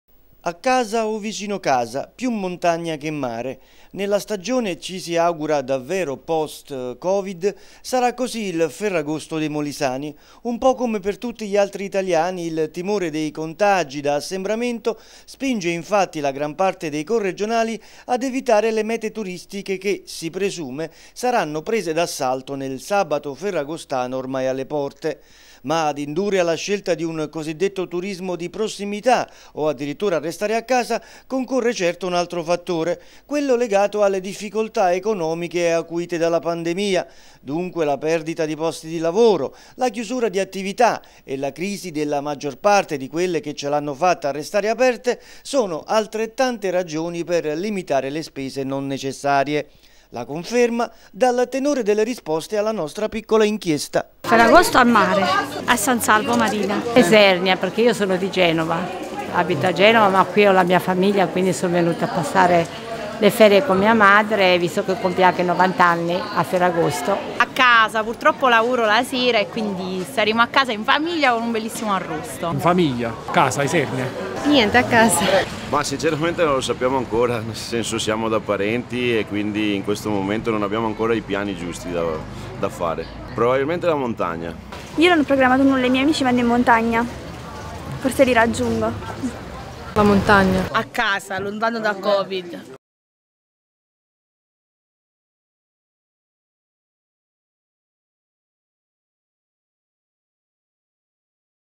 A casa o vicino casa...
Il  timore di assembramenti e le difficoltà economiche del periodo, spingono tanti Molisani a trascorrere il Ferragosto, ormai alle porte,  in modo più sobrio e un po' “al risparmio”